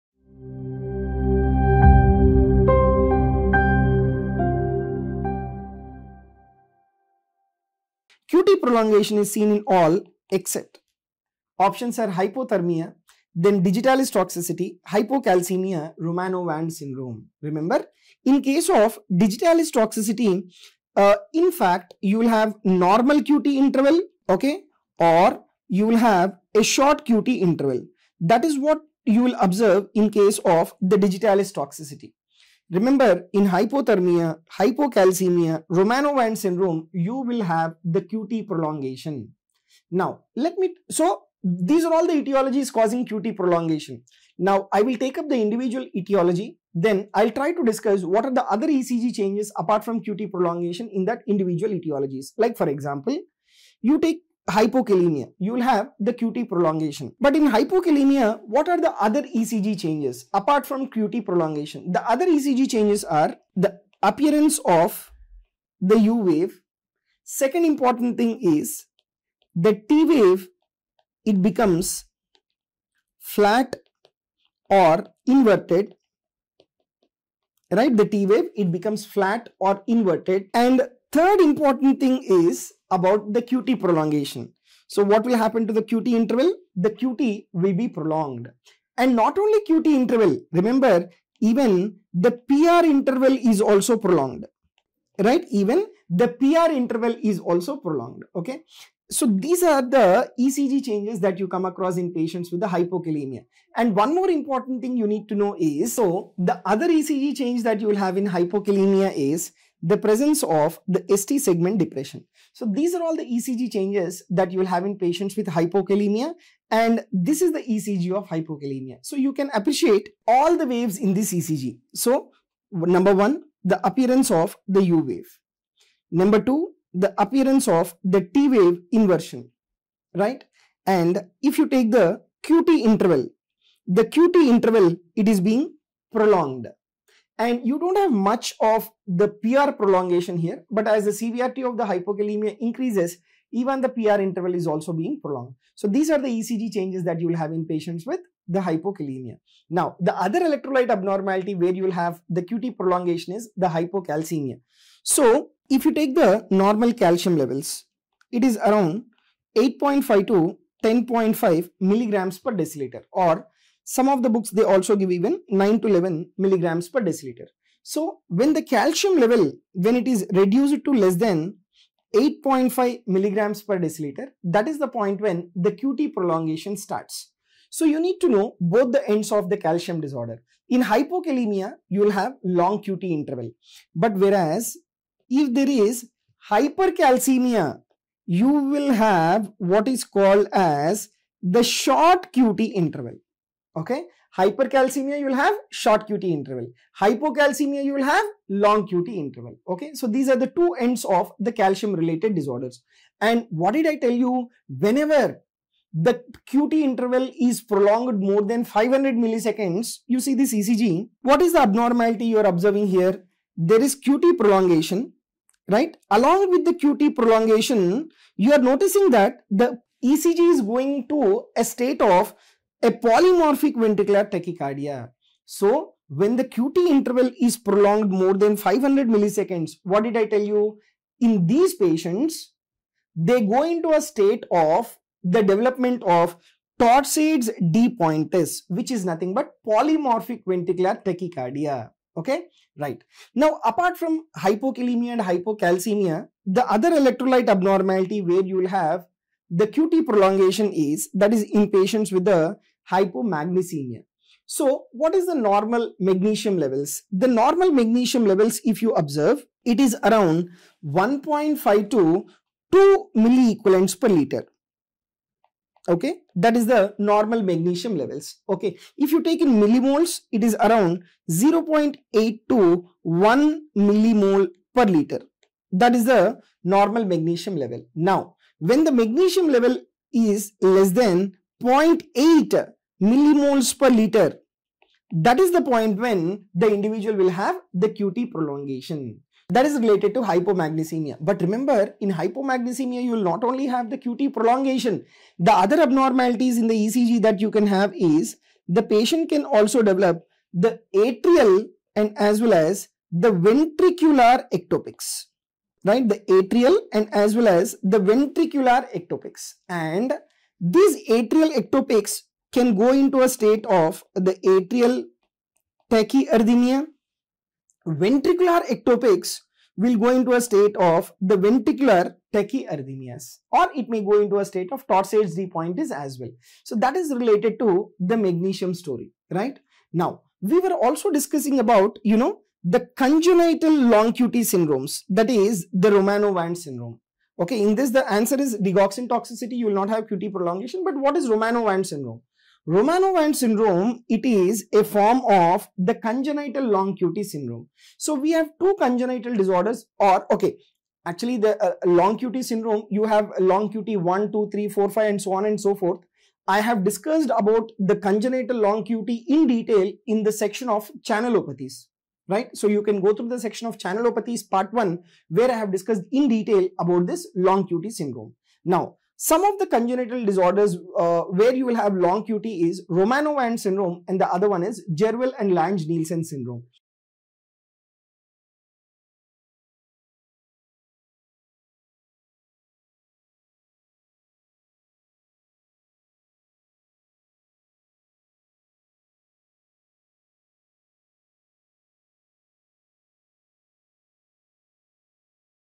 QT Prolongation by Dr. Rajesh Gubba
Watch Dr. Rajesh's video on QT Prolongation, A valuable resource for NEET Super Specialty in Medicine preparation. Gain insights into managing this condition effectively, equipping yourself with the knowledge needed for success in the exam.